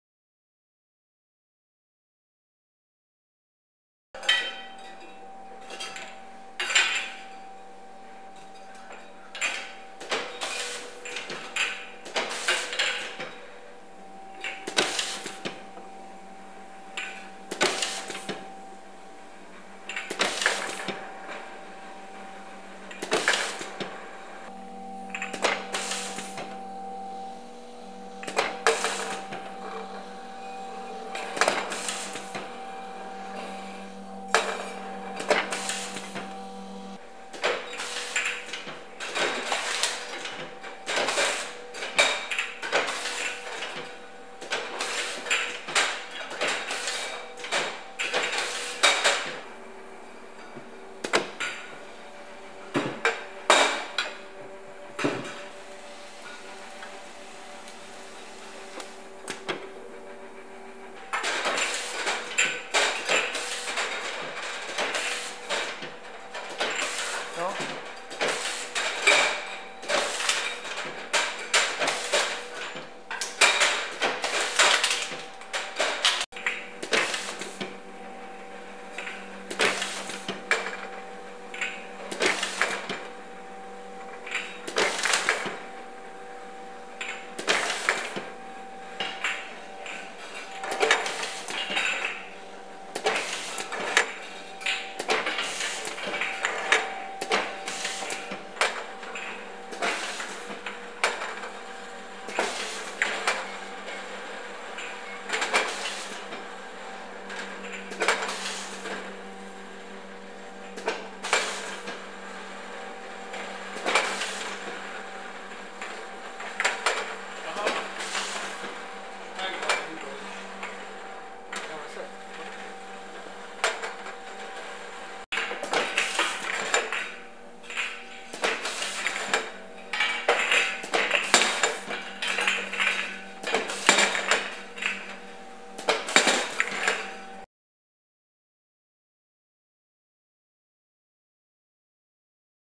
ELA-3300 wrap-around labelling for Aerosol spray
The ELA 3300 is a Hungarian-developed machine, with german Herma H400 aplicator that is capable of bottle labeling or labeling any cylinder shaped product. Suitable for double labeling tasks, such as wine bottle labeling. In special cases, it is also suitable to place a "post-positioned label" on the product to a well-defined location, such as price tags, bar codes, or labels containing information in a foreign language. In special cases, the machine can be equipped with thermal printer to print dates, barcodes, serial numbers, or to print any other information.

The diameter and height of the products can be positioned easily.Application: wine labeling, labeling of cosmetic products, labeling cans, paint cans labeling, labeling honey.
The diameter and height of the products can be positioned easily.

Application: wine labeling, labeling of cosmetic products, food or paint cans or honey labelling.